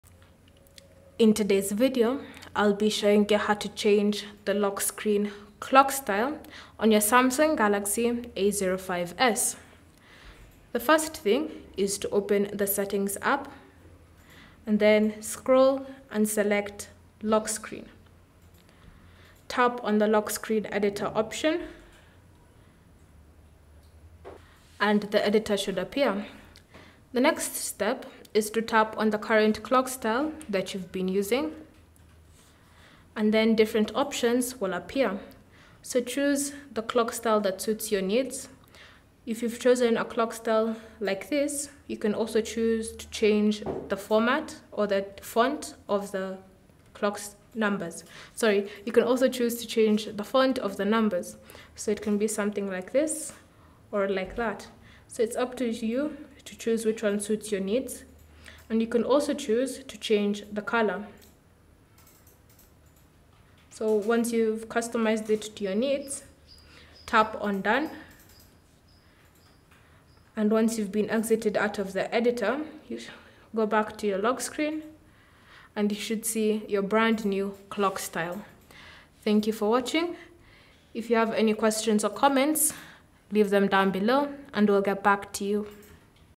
Samsung Galaxy A05s: How To Change Lock Screen Clock Style
Learn how to change the lock screen clock style on the Samsung Galaxy A05s

The Samsung Galaxy A05s comes with a variety of customization options, allowing users to personalize their devices according to their preferences. One such customization feature is the ability to change the lock screen clock style.

This can be a fun and practical way to give your device a unique look and feel. In this tutorial, we'll guide you through the steps to change the lock screen clock style on your Samsung Galaxy A05s.

To Change Lock Screen Clock Style On Galaxy A05s
Select Lock Screen
Select Edit under the lock screen widget
Tap on your clock
Choose different clock style
Tap on Done

Galaxy S23 Ultra DUAL SIM 256GB 8GB Phantom Black:

Cell Phone Tripod Adapter